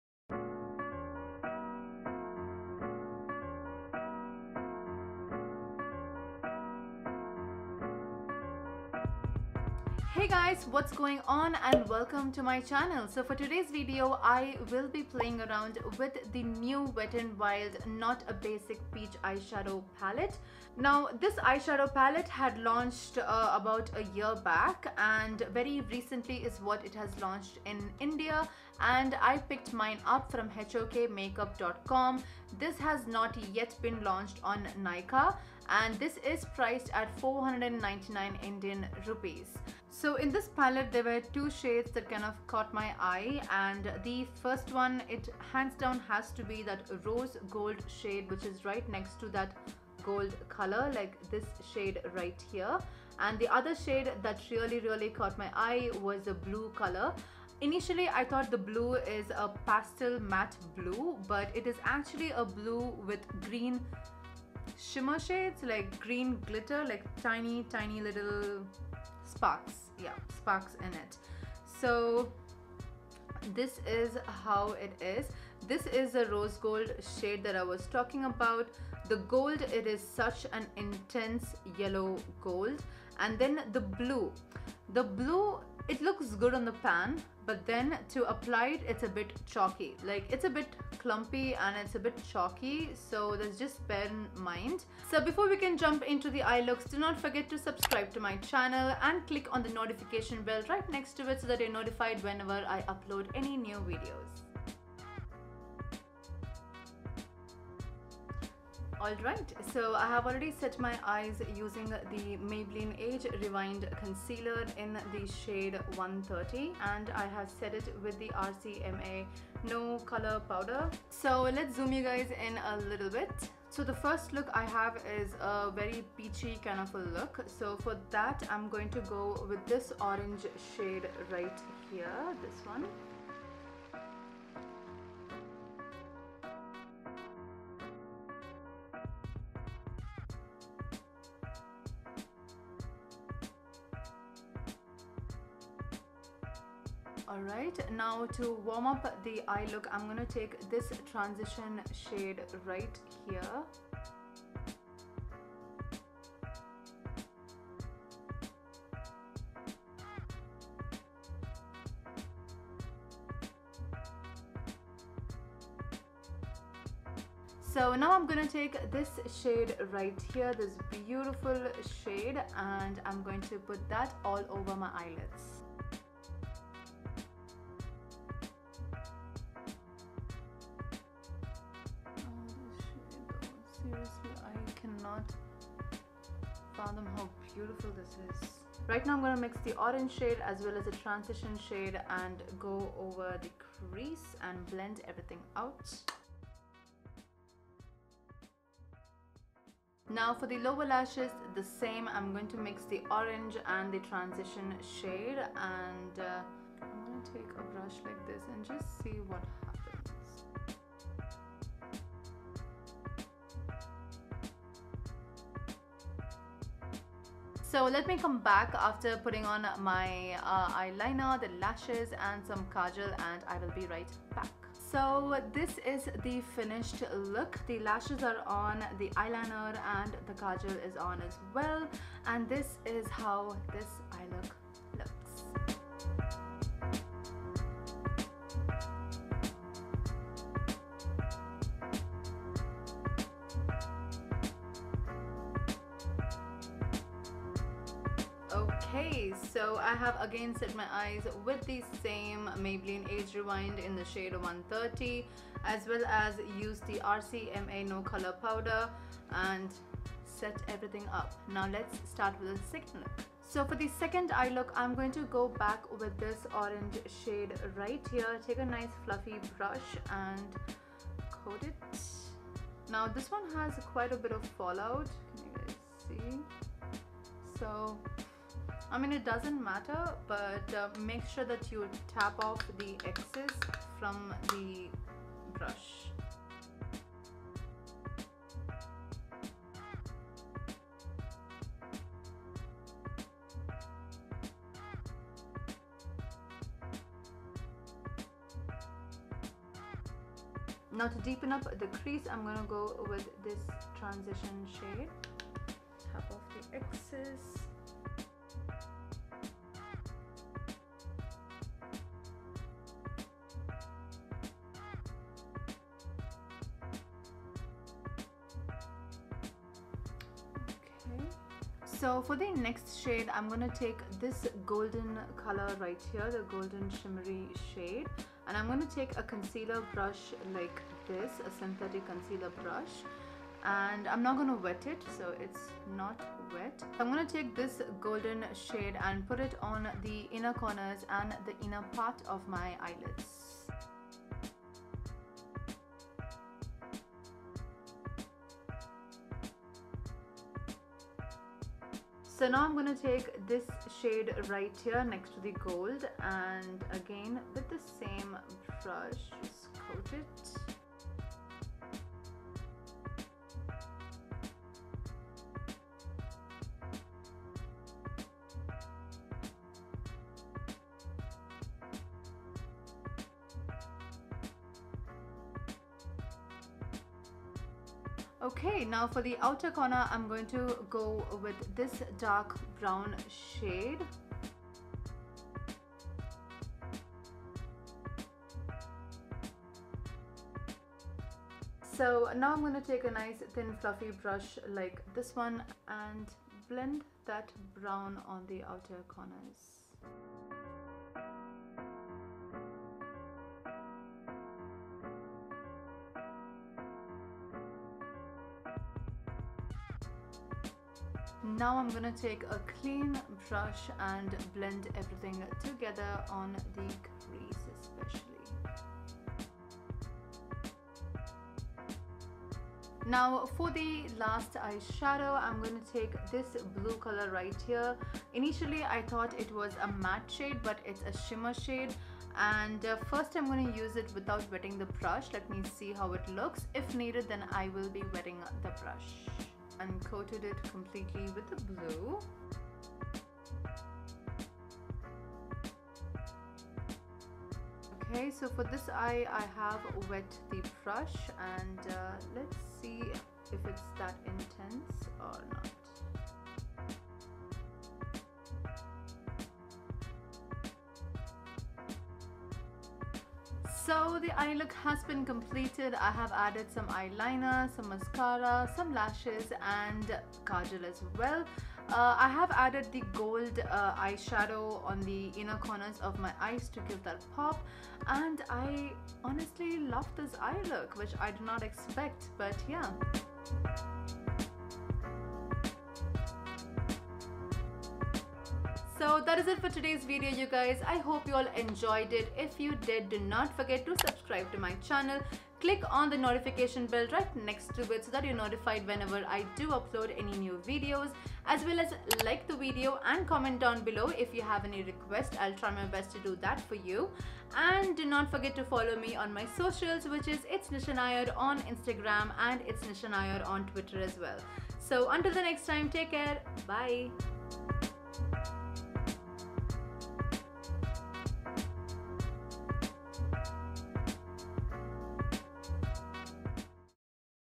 NEW Wet N Wild Not a Basic Peach Palette India || Two Looks One Palette
Hey Guys,

Here is a video where I created two looks using the New Wet n Wild Not a Basic Peach Palette.

Lipstick: Colourpop Ultra Matte in Speed Dial